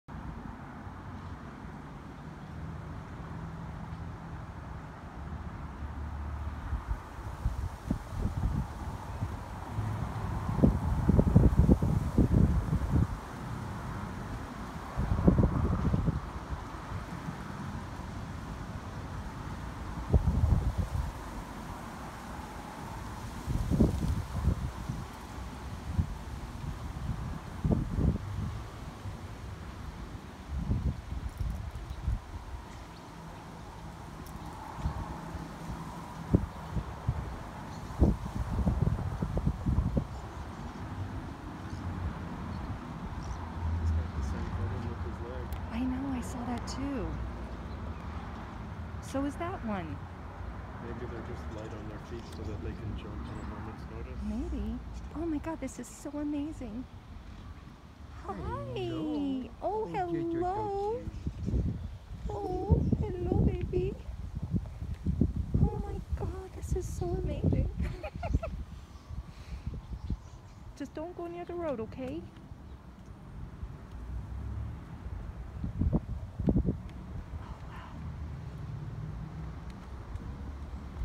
Deer- Gunnison Colorado
Deer outside Optometrist office in Gunnison, Colorado